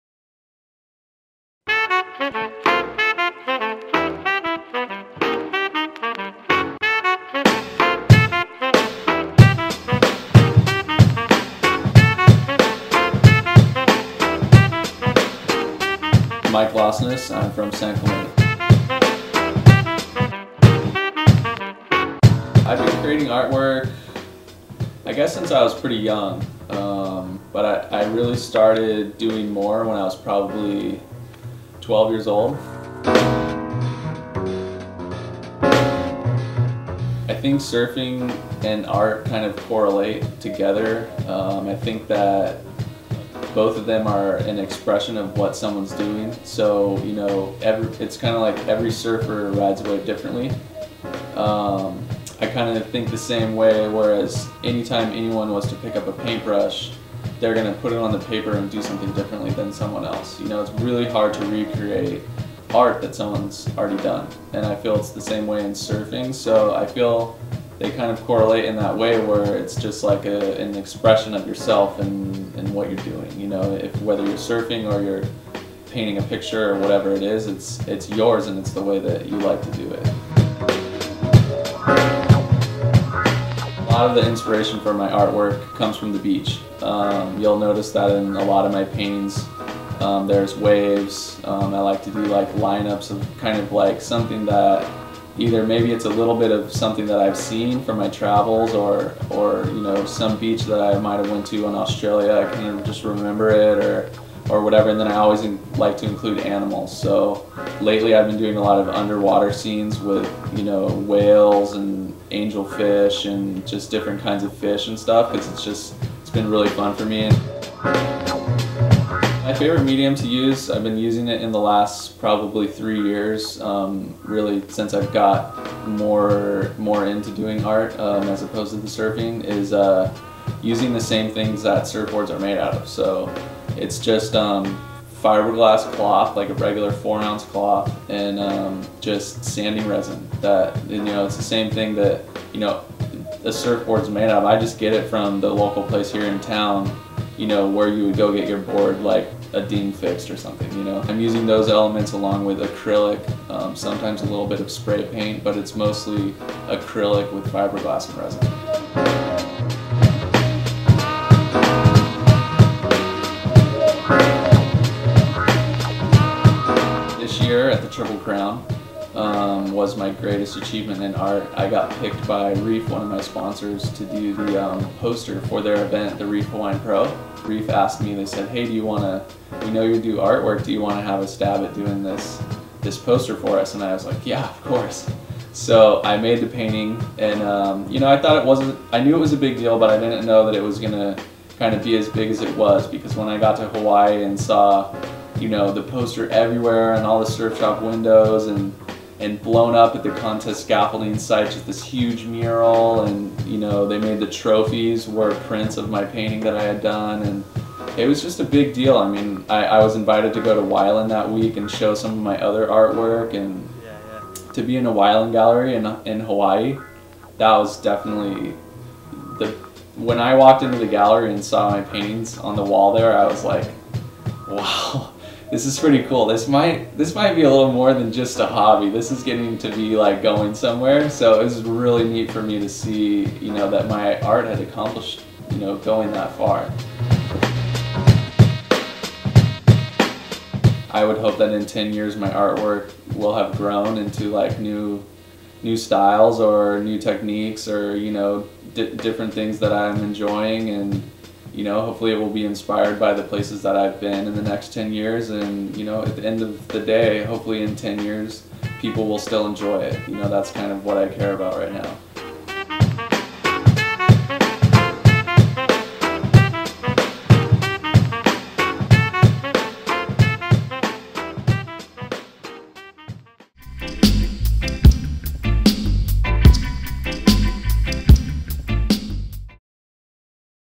Mike Losness interview
Mike is more than a good surfer, he's also and great artist

Film/Edit Matt Wybenga